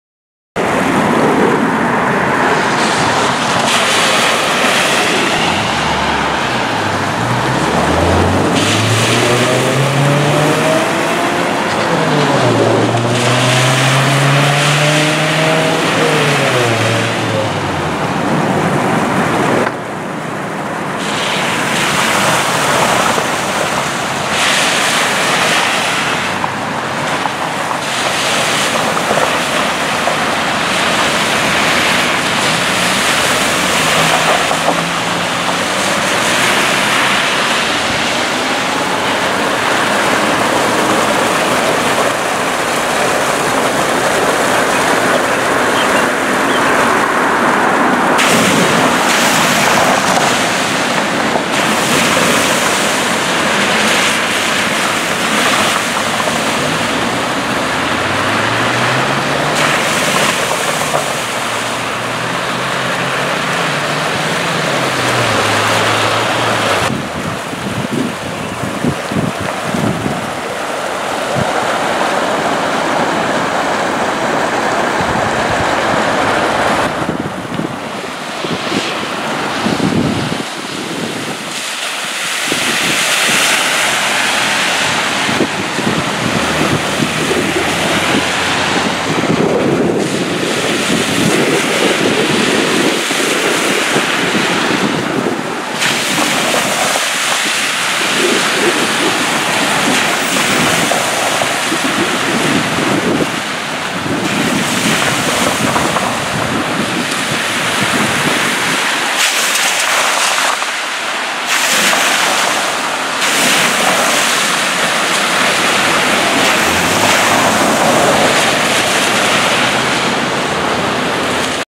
water main broke
June 2 2015 i came home to water running out of the ground